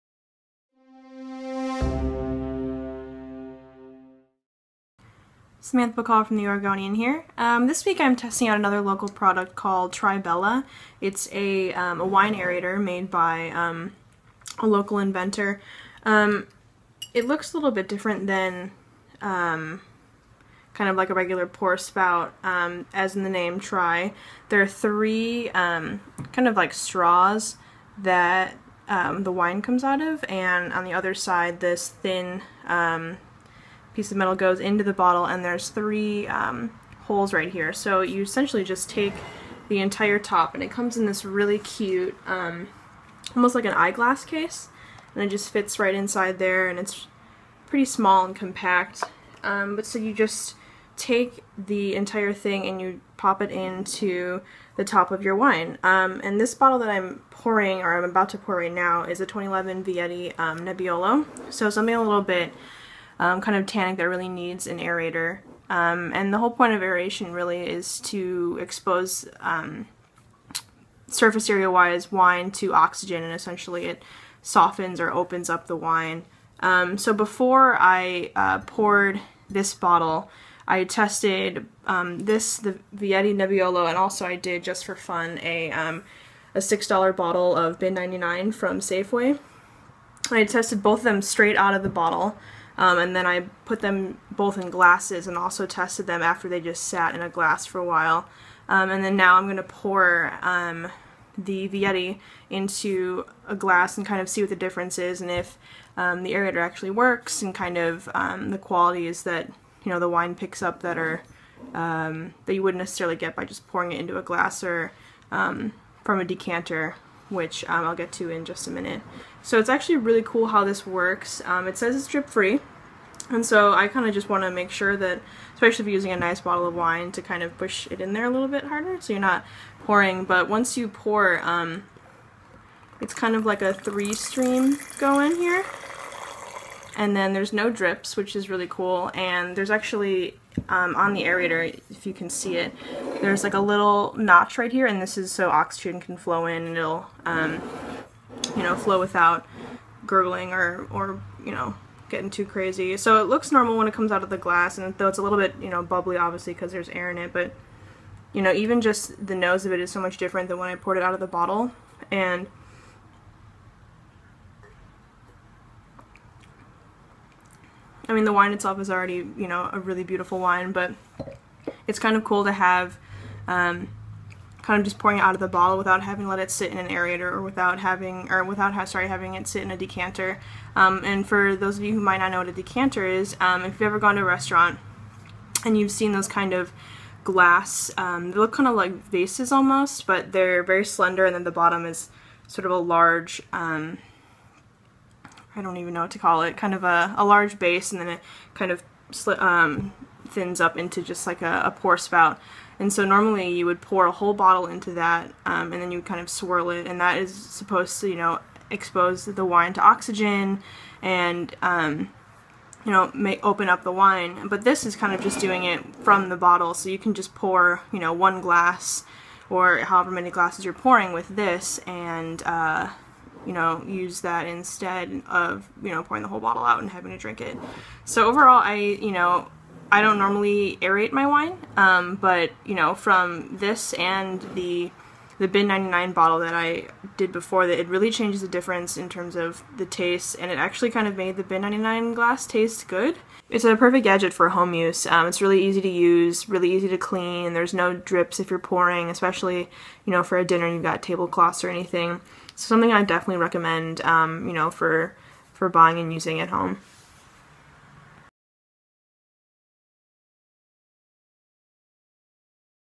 Tribella Wine Aerator
I test out a locally created wine aerator on a bottle of 2011 Nebbiolo and a $6 Pinot Noir to see how it works.